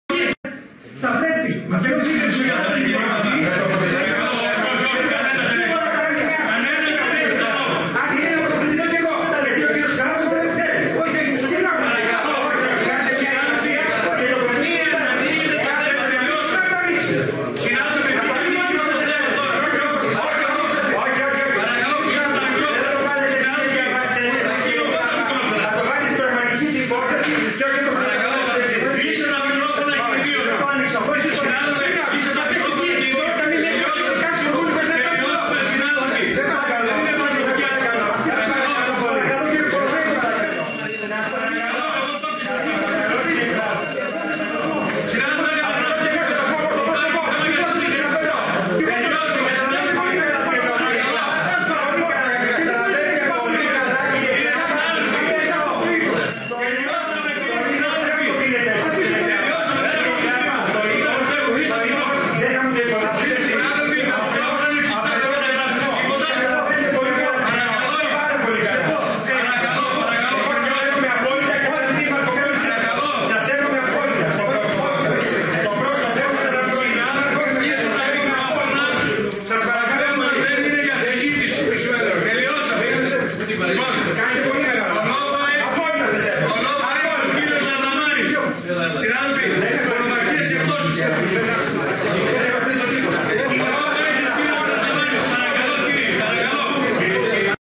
Ένταση μεταξύ Κορωναίου - Κλάπα στο ΔΣ
Ένταση Κορωναίου Κλάπα στο ΔΣ για τις εγγυητικές του Λιμενικού ταμείου